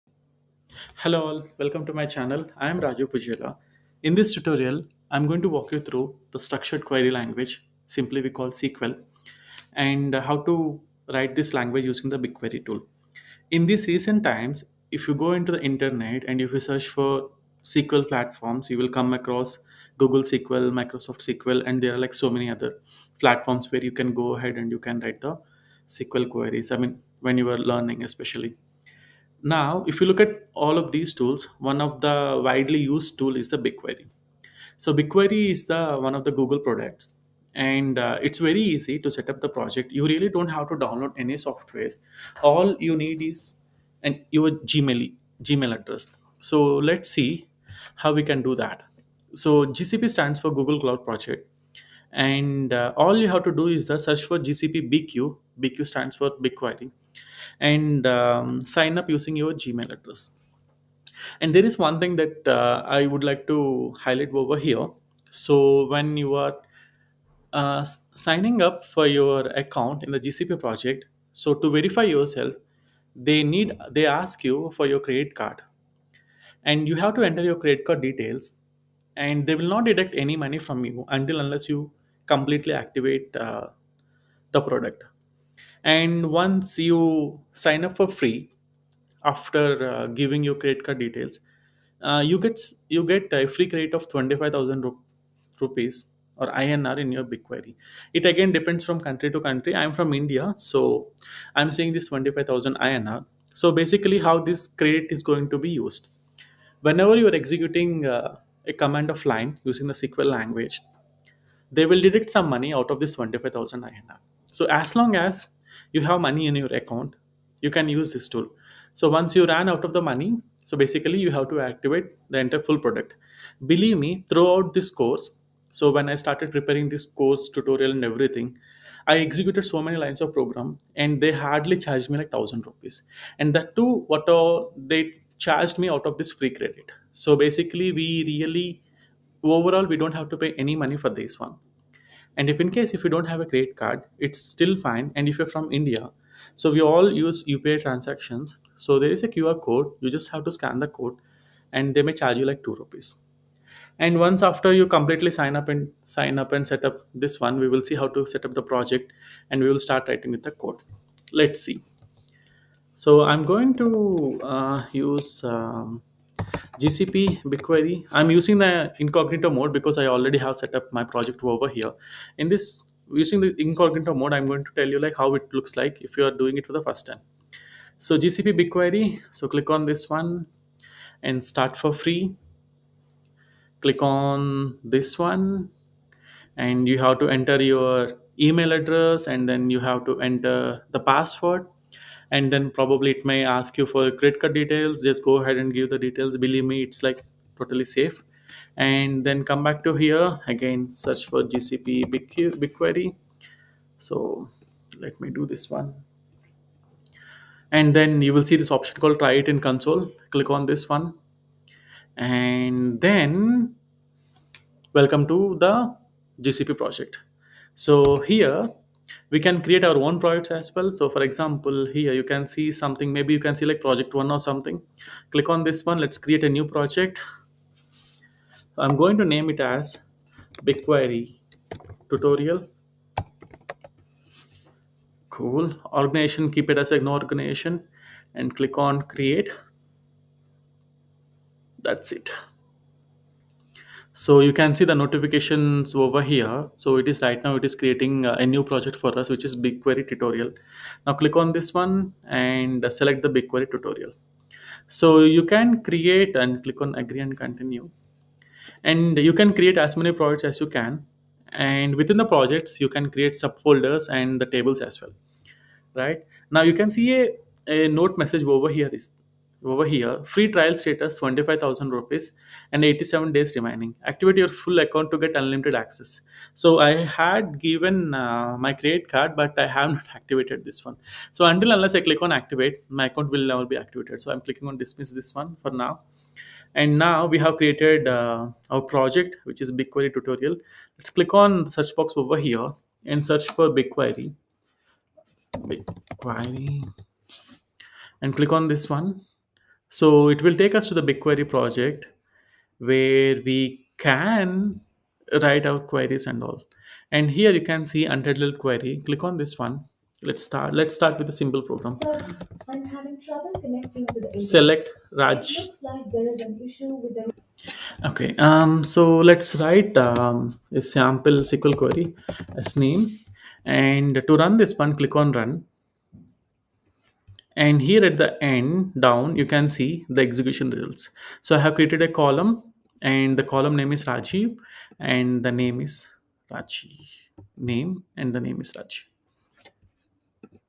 1. How to Set Up a Google Cloud BigQuery Project | Step by Step Tutorial
Learn how to set up your first Google Cloud BigQuery project in this step-by-step tutorial. 🚀 Whether you're new to BigQuery or want a refresher, this video covers everything you need to know to get started, including:

✔️ Creating a Google Cloud Platform (GCP) project
✔️ Enabling the BigQuery API
✔️ Setting up billing and permissions
✔️ Navigating the BigQuery console

BigQuery is a powerful tool for analyzing massive datasets, and this tutorial will help you set the foundation for all your data analytics projects.